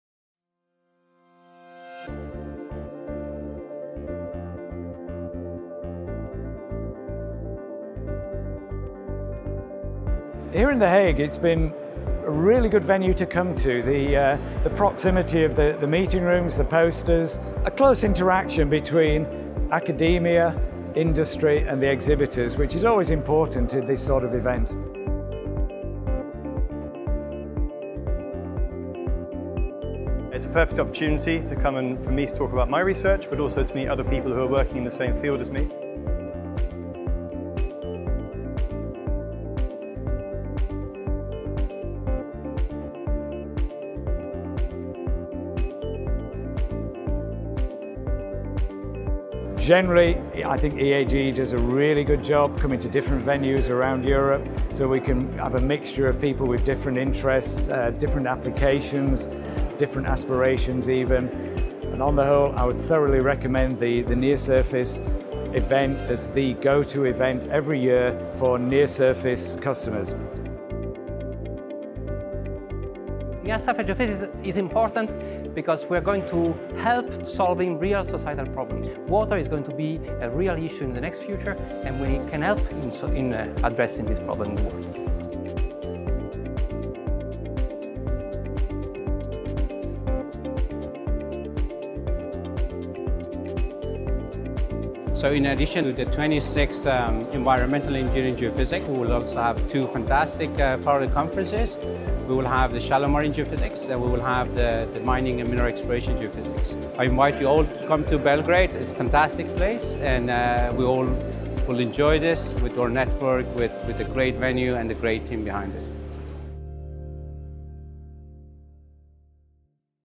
NSG 2019 After Movie
A big thank you to everyone that joined us in The Hague for the 2019 edition of the Near Surface Geoscience Conference & Exhibition!

At NSG, participants enjoy a unique platform to exchange knowledge on the recent developments in near-surface geosciences, on breakthroughs in geophysical exploration, assessment of geothermal energy and underground storage of renewable energy, and on geophysics applied to underground infrastructure projects. During these meetings, field trips, workshops, and instrument and field method demonstrations take place.

Join us next year to have the full Near Surface Geoscience Conference & Exhibition experience! for more details.